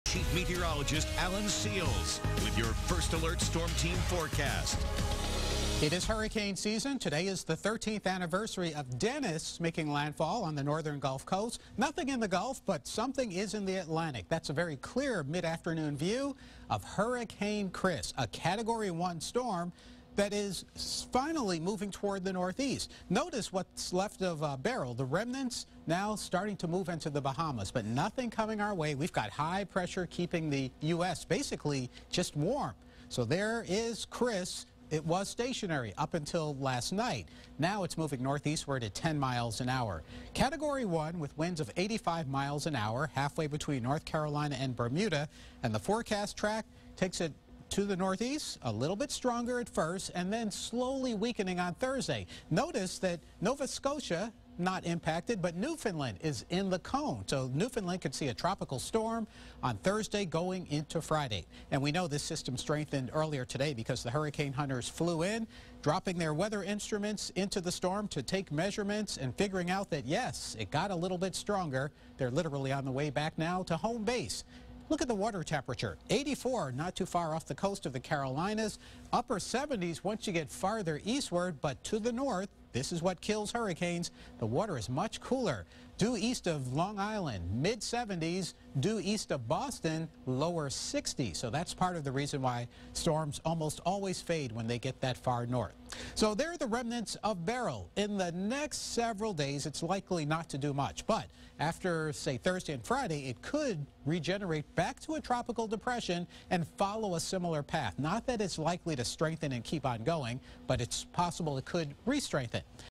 Category 1 Hurricane Chris Moves Northeastward
Hurricane Chris formed today from what was Tropical Storm Chris. With winds of 85mph, the storm is between North Carolina and Bermuda.

The motion will keep it away from the US East coast and from Bermuda. They will get rip currents and swells. Chris will likely get a little stronger tomorrow before weakening in the colder waters of the northern Atlantic. Toward the weekend, it may hit Newfoundland as a tropical storm.

Chris is the second hurricane of the 2018 hurricane season, and the third named storm of the season. Follow the track on your free WKRG News 5 weather app.

Remnants of Beryl may redevelop into a depression over or near the Bahamas in a few days as they move northward in the Atlantic